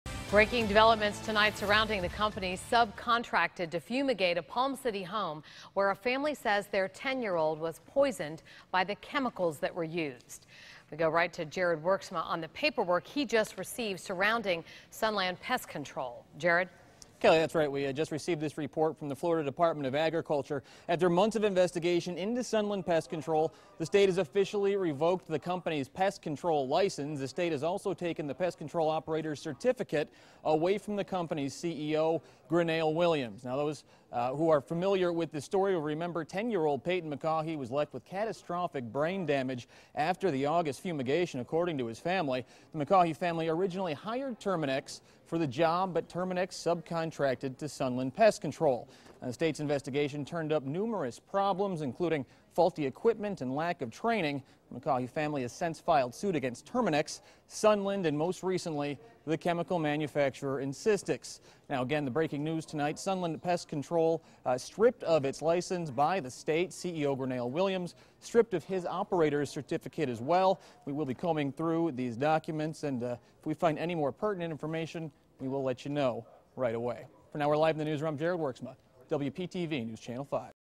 License revoked for Sunland Pest Control
Company subcontracted to fumigate Palm City home